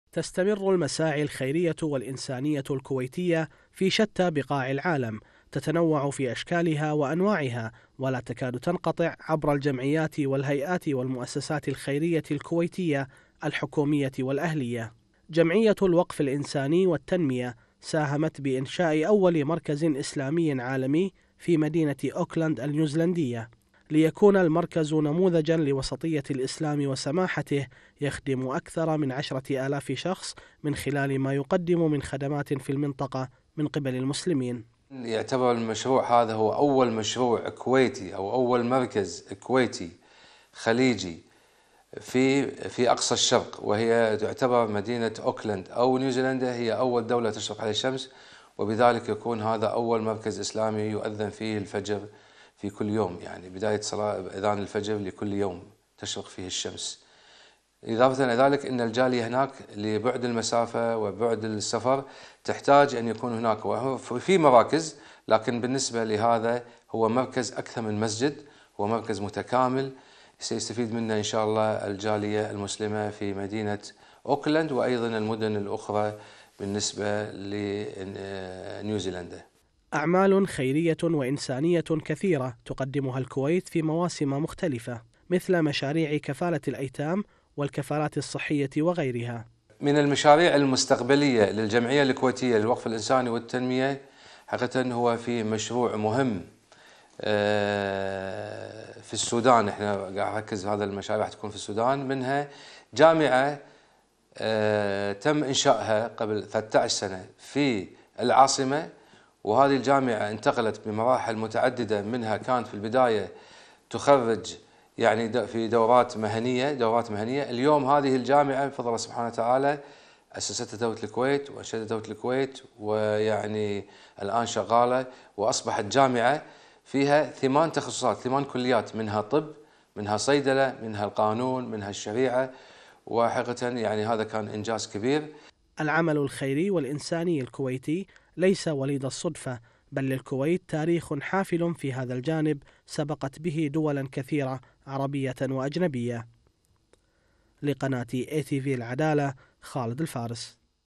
"الكويتية للوقف الإنساني" دشنت أول مركز إسلامي في نيوزيلندا
نصار العبدالجليل
رئيس الجمعية الكويتية للوقف الإنساني والتنمية

تطبيق